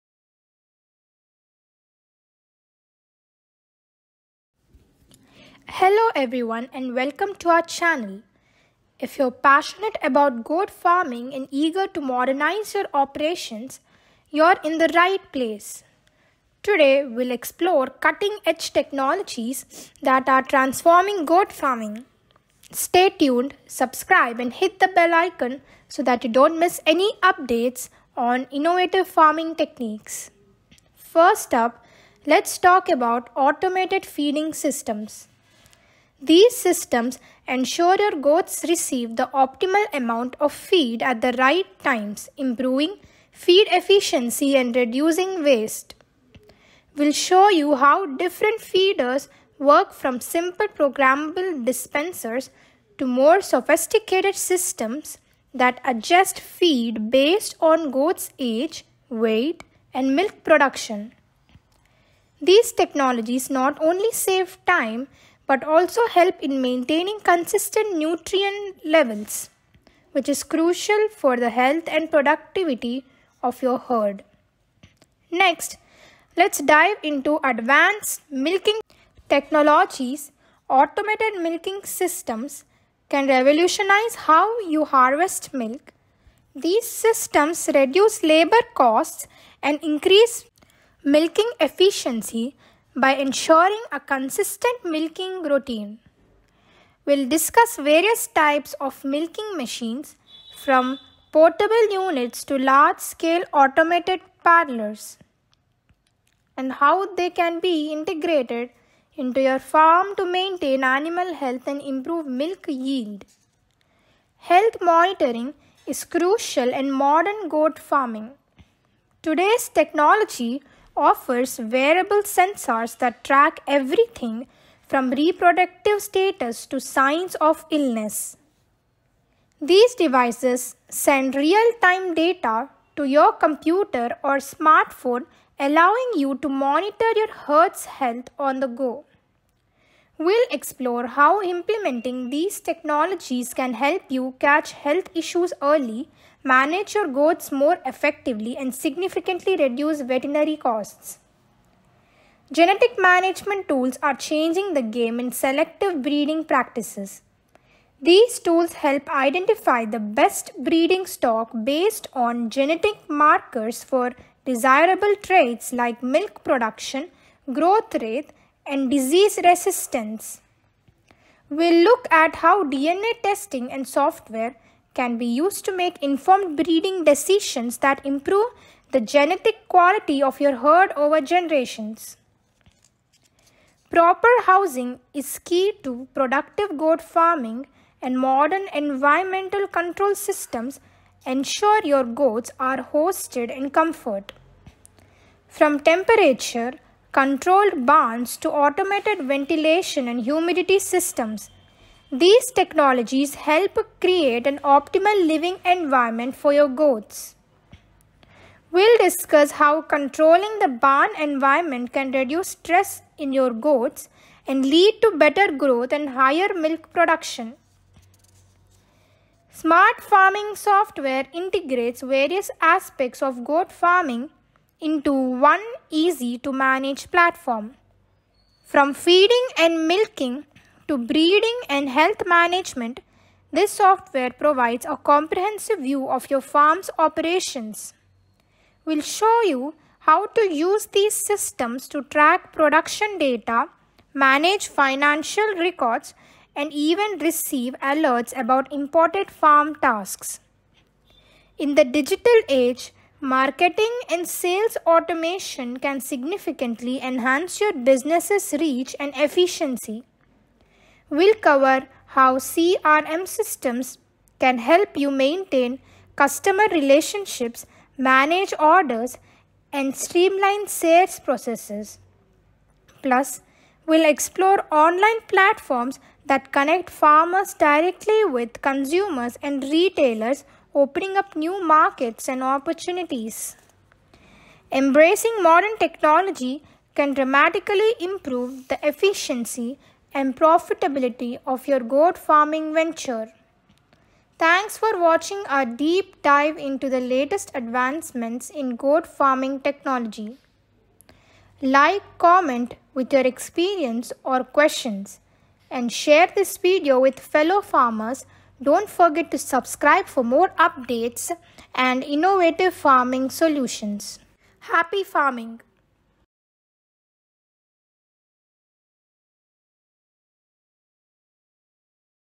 Modern Goat Farming Technology | Agri Farming
Step into the future of goat farming with our comprehensive video on modern goat farming technologies! From automated feeding systems and advanced milking setups to sophisticated health monitoring and smart farming applications, we cover the latest innovations that are revolutionizing goat farming. Ideal for both new and experienced farmers, this video provides insights into the tools and techniques that can significantly enhance the efficiency and productivity of your goat farming operation.

What You Will Learn:

Automated Feeding Systems: Explore how these systems can manage diet and improve feed efficiency.
Advanced Milking Setups: Discover methods to optimize milking processes and improve milk yield.
Health Monitoring Systems: Learn about technologies that track the health and welfare of your goats in real time.
Smart Farming Applications: Understand how integrating data-driven software can streamline your farm operations.